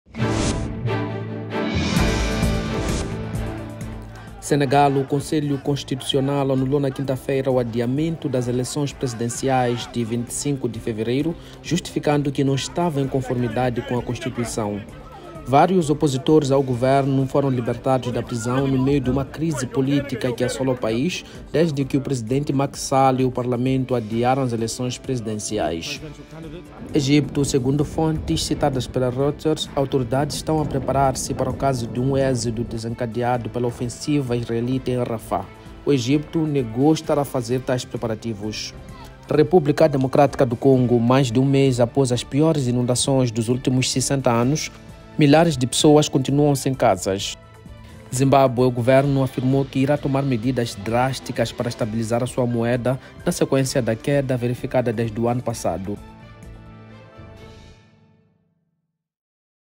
Manchetes africanas: Senegal - Várias figuras da oposição foram libertadas
Senegal: O Conselho Constitucional anulou na quinta-feira o adiamento das das eleições presidenciais.
República Democrática do Congo: Mais de um mês após as piores inundações dos últimos 60 anos, milhares de pessoas continuam sem casa.
Zimbabwe: Em meio a crise, 80% das transacções no mercado são agora feitas em dólares americanos.